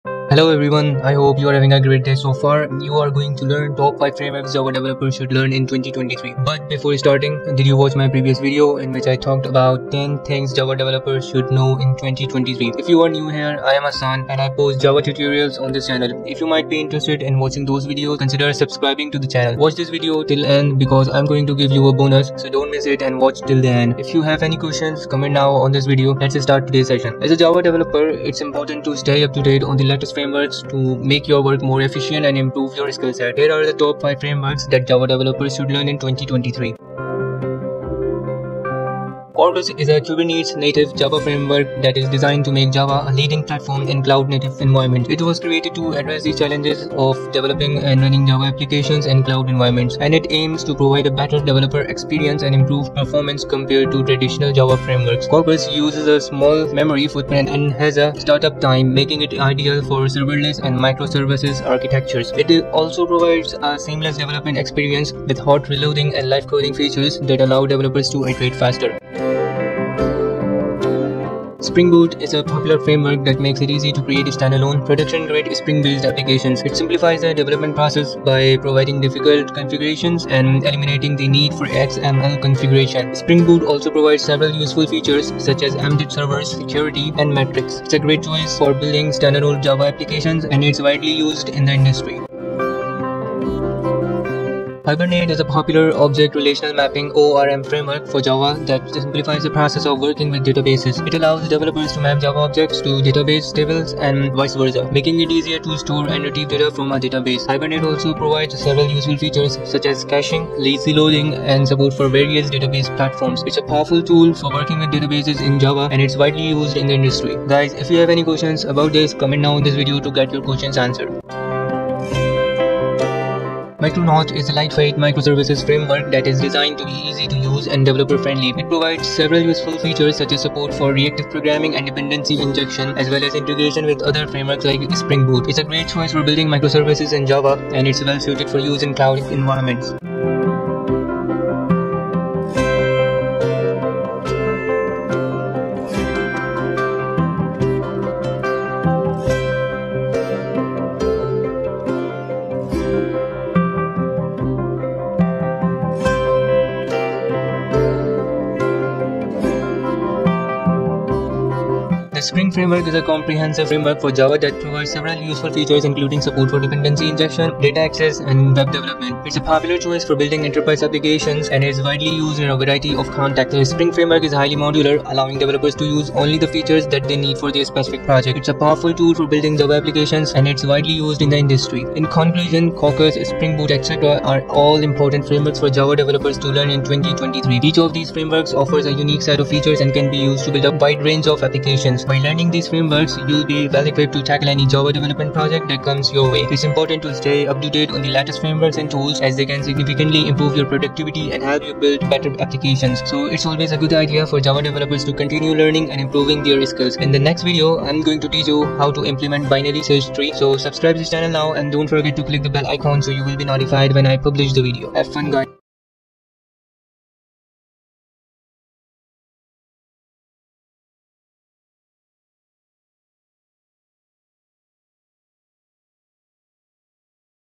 Top 5 Framework and Libraries Java Developer Should Know
For building web applications, there are numerous Java frameworks available, each with its own advantages and applications. Quarkus is a brand-new lightweight container-first framework designed specifically for building serverless applications and microservices.

In contrast, Spring Boot is a well-liked and widely used framework for building enterprise-scale applications on a large scale. Micronaut is a framework designed specifically for developing serverless, cloud-native applications, whereas Hibernate is an object-relational mapping (ORM) framework that offers a method of database interaction that is both simple and effective.

Last but not least, the Spring Framework is a comprehensive framework with a wide range of features and tools for building web applications, including MVC, AOP, and dependency injection. In the end, the development team's preferences and experience, in addition to the application's specific requirements, determine which framework is used.

1. Quarkus (0:45)
2. Spring Boot (1:24)
3. Hibernate (1:52)
4. Mirconaut (2:26)
5. Spring Framework (3:13)